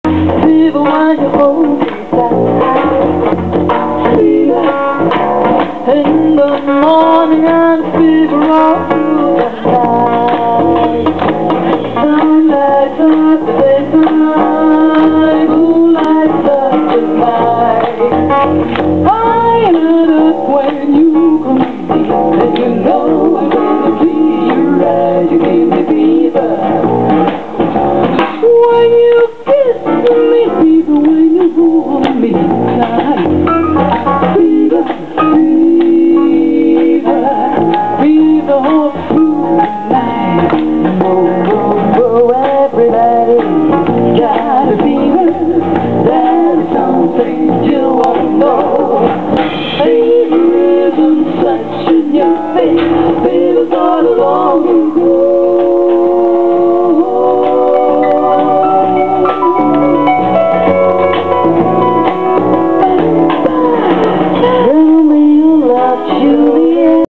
festival de Jazz In Marciac les Andrews Sisters la Nuit grand moment de musique et de danse
peu ont apprécié et pourtant pour danser,ces chants et ces musiques(qui nous ramènent  aux années folles  de GLENN MILLER   ET DES ANDREWS SISTERS ) c'était génial,et dans mon cœur cela est au chaud